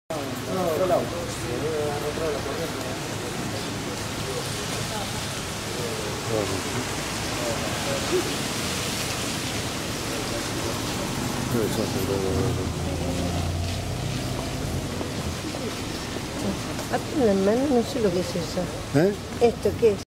Tormenta en la LA PLATA
Después que bajo el agua encontramos un cuerpo sin vida, arrastrado por el agua.